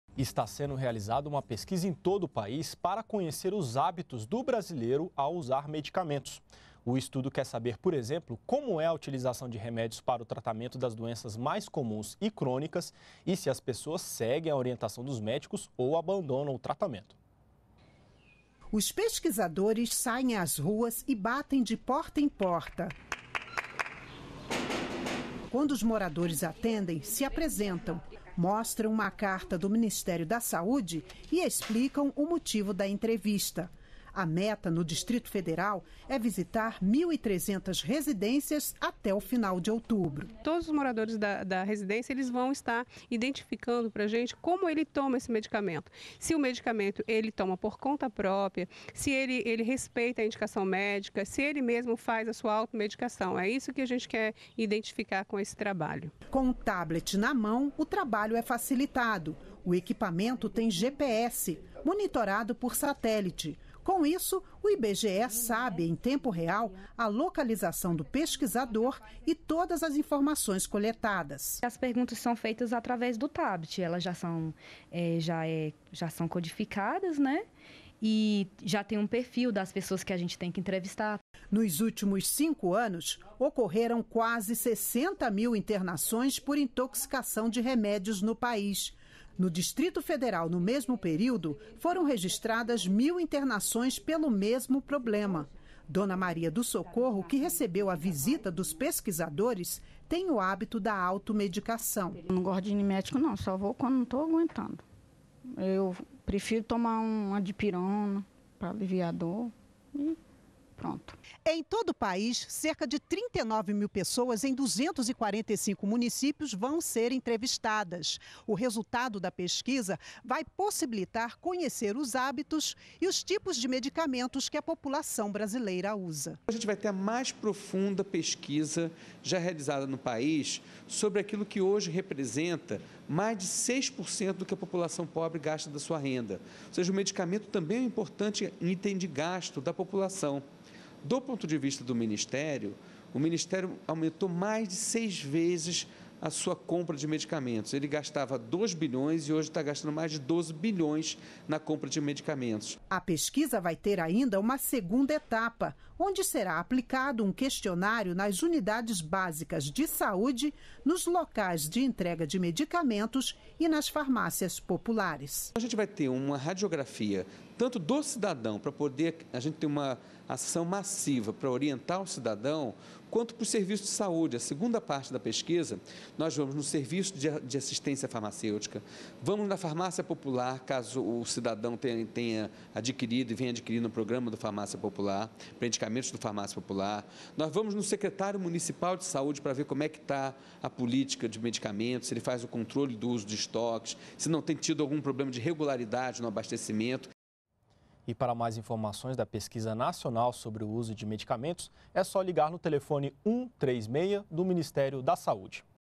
Pesquisa coleta dados sobre como a população usa os medicamentos
NBR NOTÍCIAS - 02.10.13: O Ministério da Saúde iniciou a coleta de dados da Pesquisa Nacional sobre Acesso, Utilização e Promoção do Uso Racional de Medicamentos (Pnaum). Em todo o país cerca de 39 mil pessoas em 245 municípios vão ser entrevistadas. Na primeira etapa, a pesquisa vai para conhecer os hábitos do brasileiro e os tipos de medicamentos mais usados pela população. Na segunda fase, será aplicado um questionário nas unidades básicas de saúde, nos locais de entrega de medicamentos e nas farmácias populares. Informações pelo telefone 136 ou na página